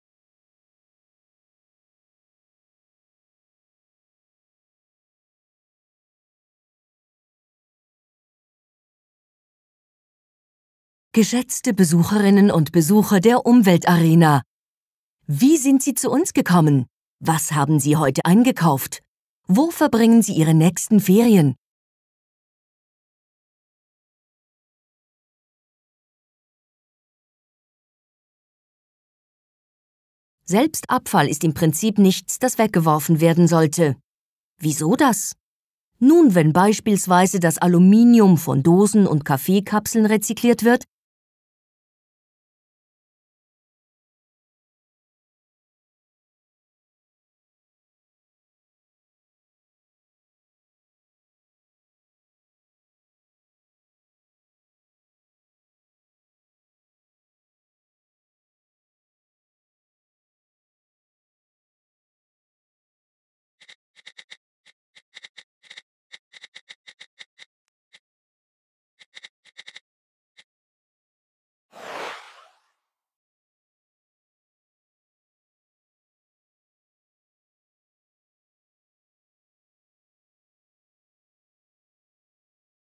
App for Umwelt Arena Spreitenbach: audio tour relating to the topic of sustainable consumption
How can I reduce my own water consumption by taking simple measures? How big is my ecological footprint? Why are ecological labels so important? In Umwelt Arena Spreitenbach, 120 firms and organisations show, in 45 interactive exhibitions, how everyday life can be structured to be more sustainable without any loss of comfort. At 16 selected stations, the smartphone app provides explanations, tips and valuable information in the form of text, images and sound. The Umwelt Arena app was developed with the help of the Tour App Maker from Audiotours GmbH. With this web-based Content Management System, app contents can be created and edited in a cost-effective and time-saving manner.